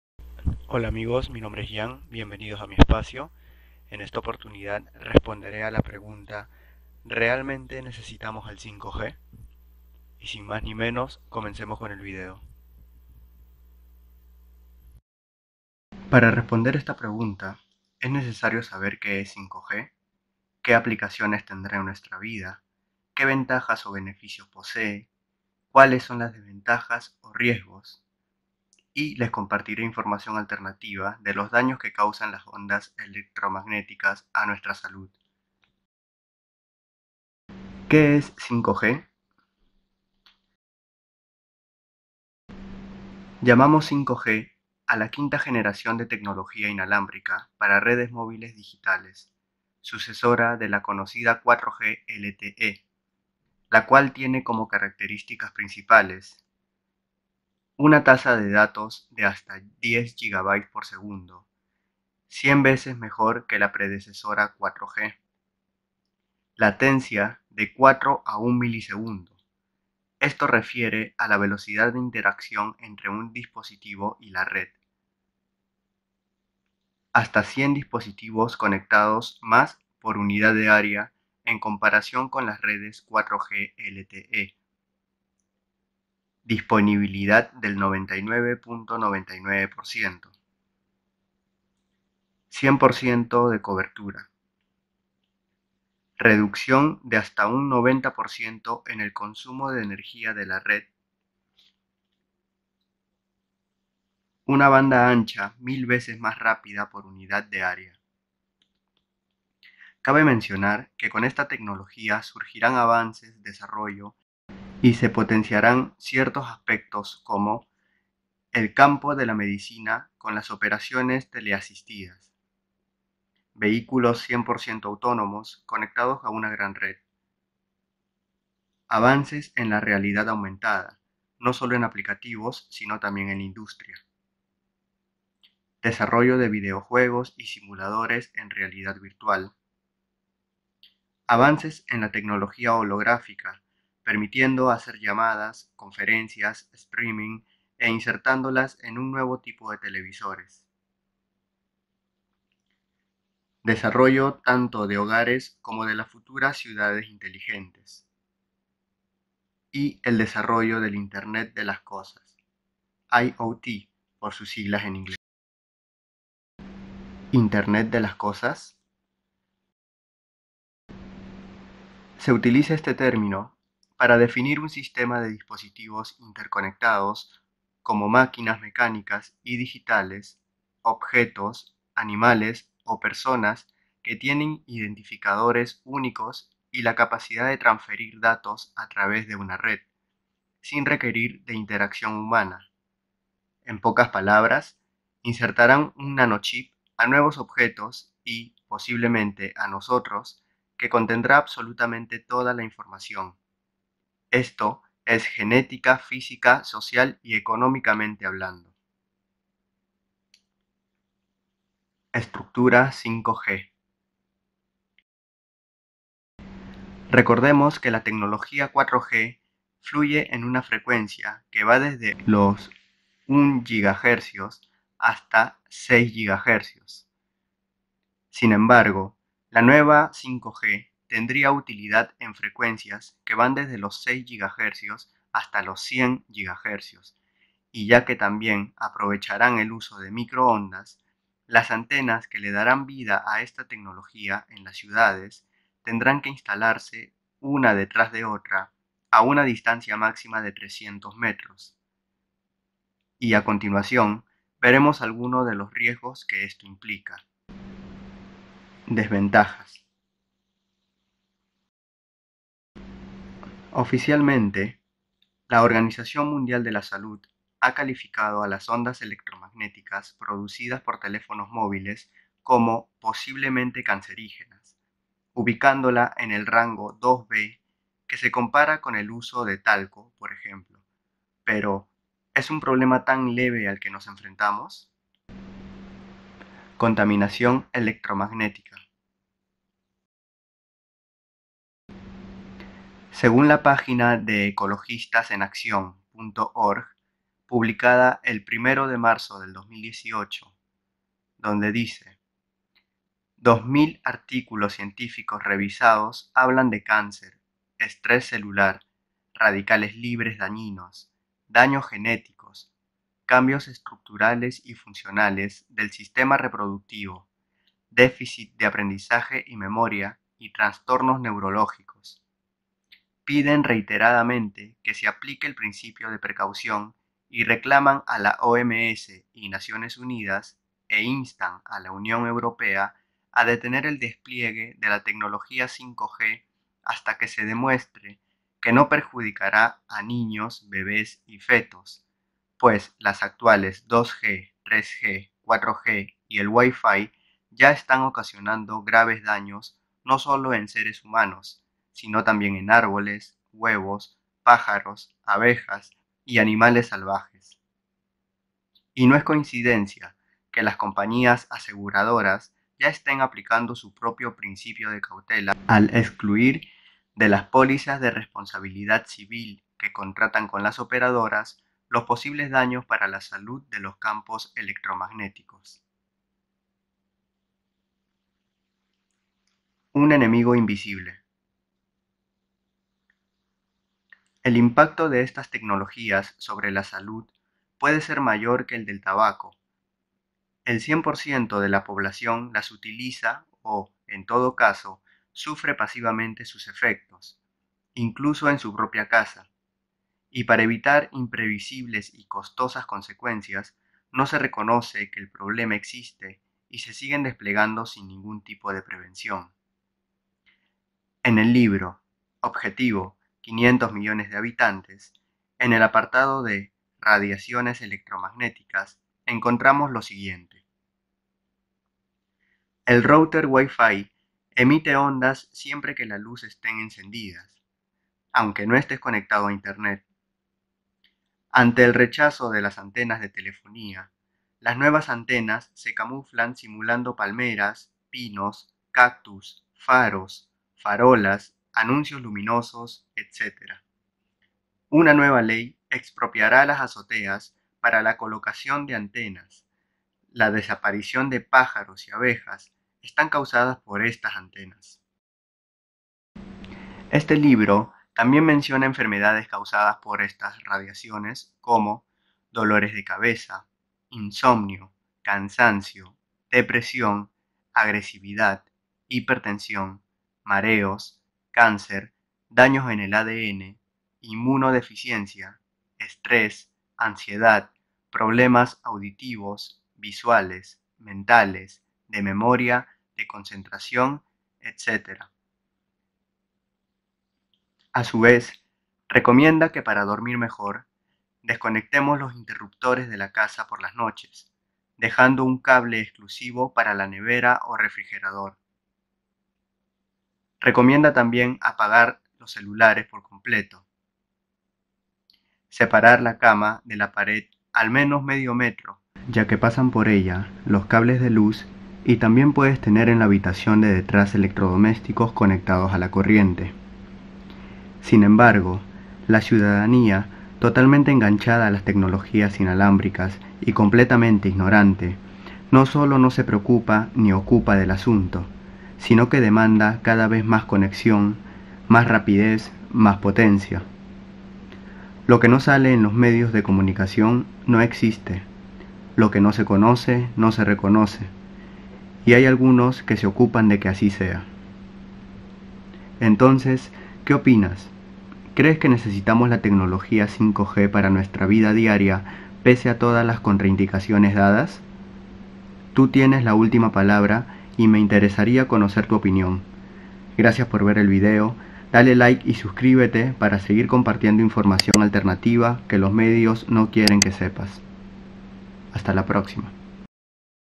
REALMENTE NECESITAMOS 5G ? | TU OPINIÓN IMPORTA.
Hola a Todos, en este vídeo abordaremos un tema que conforme va pasando el tiempo se vuelve mas polémico, entraremos en definición, características, ventajas, desventajas y riesgos que contiene esta nueva tecnología, pero lo que mas me importa es tu opinión... CREES QUE REALMENTE NECESITAMOS 5G, tu tienes la ultima palabra, saludos.